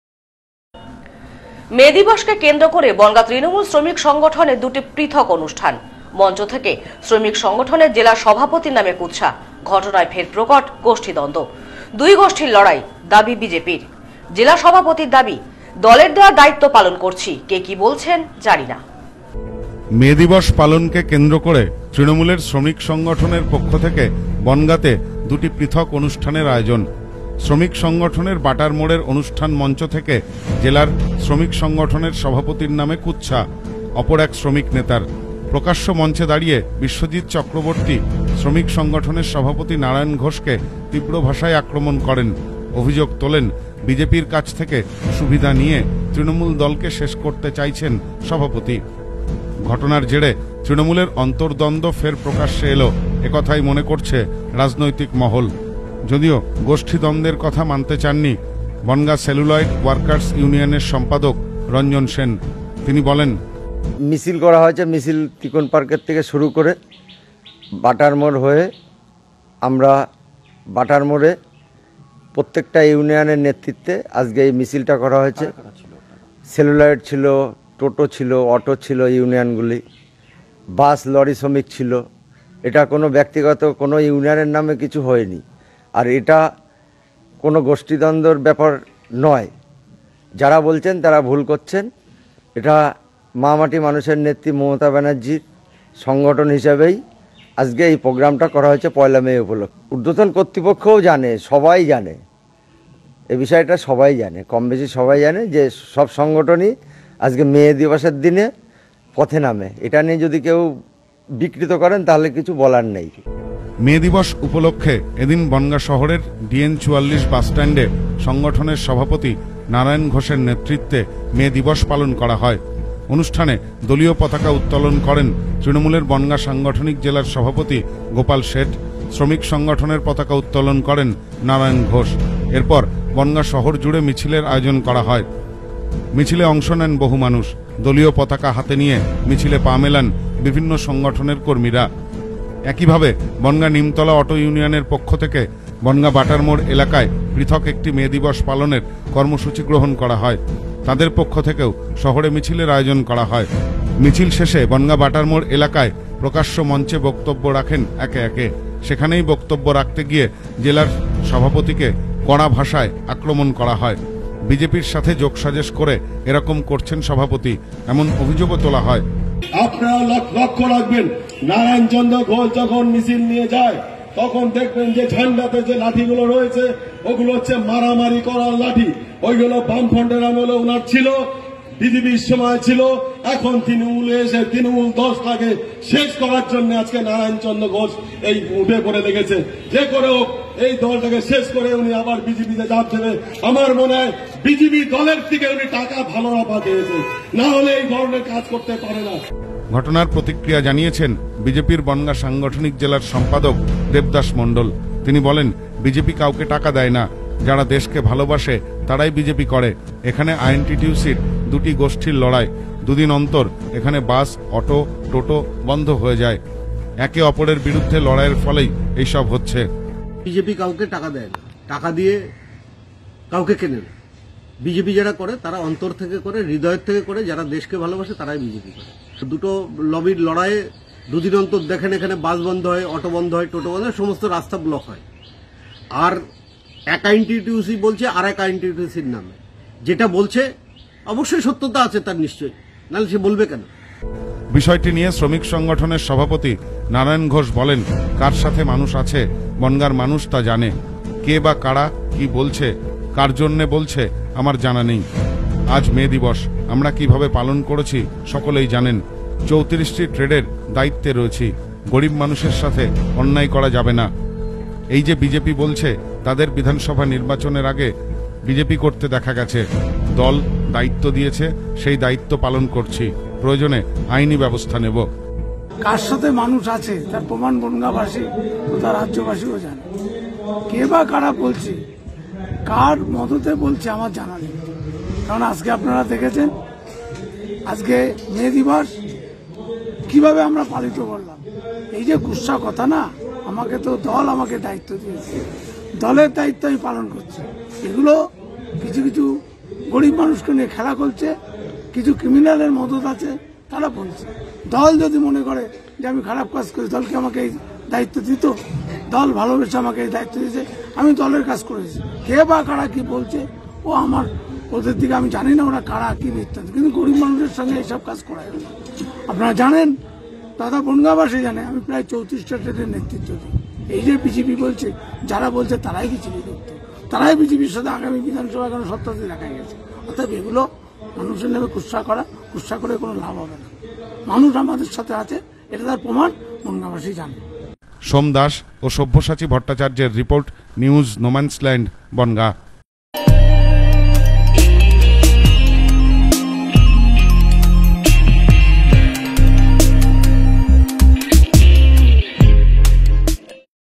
মে দিবস পালন কেন্দ্র করে বনগাঁ তৃণমূল শ্রমিক সংগঠনের দুটি পৃথক অনুষ্ঠান।
মে দিবস পালন কেন্দ্র করে বনগাঁ তৃণমূল শ্রমিক সংগঠনের দুটি পৃথক অনুষ্ঠান। অনুষ্ঠান মঞ্চ থেকে শ্রমিক সংগঠনের জেলা সভাপতির নামে কুৎসা। ঘটনায় ফের প্রকট গোষ্ঠীদ্বন্দ্ব। দুই গোষ্ঠীর লড়াই, দাবি বিজেপির। জেলা সভাপতির দাবি, 'দলের দেওয়া দায়িত্ব পালন করছি। কে কি বলছে জানা নেই।